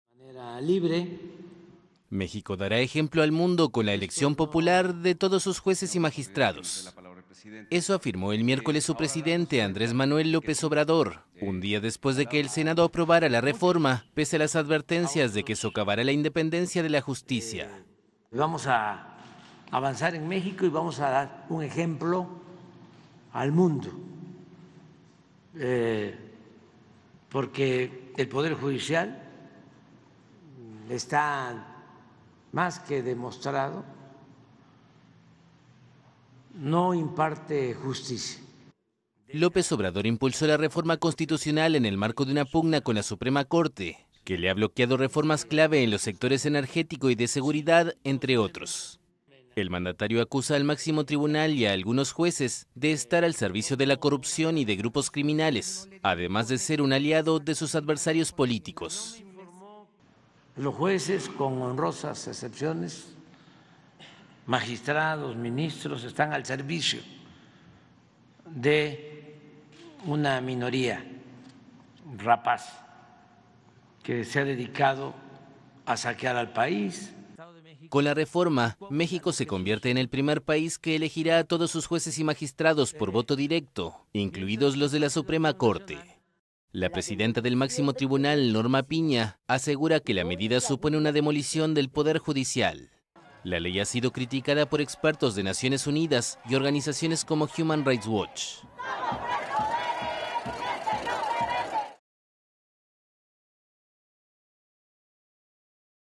López Obrador afirmó que México dará un "ejemplo al mundo" con la elección popular de jueces
El presidente de México, Andrés Manuel López Obrador, aseguró que su país dará un "ejemplo al mundo" con la elección de todos sus jueces y magistrados por voto popular, pese a las advertencias de que socavará la independencia de la justicia.